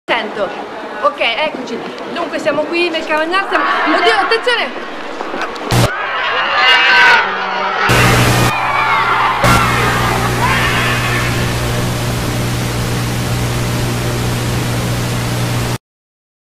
INFEZIONE IN UN QUARTIERE DI VALENCIA - CABANYAL
La TV italiana riporta l'infezione origine nel Cabanyal. C'è la paura che l'infezione si diffonde attraverso l'Europa. Il giornalista è stato il suo ultimo giorno di contratto di lavoro, un vero peccato.